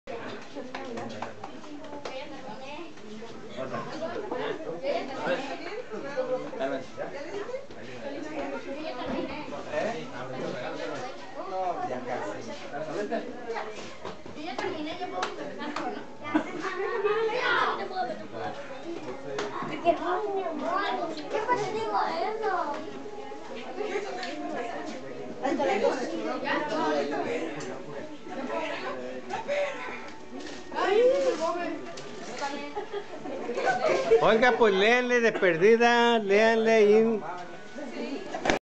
Cena de Navidad 2014 En Casa de Joaquin Jr. HD
Cena donde reino la alegría, los deliciosos bocadillos
en compañía de nuestros hijos y nietos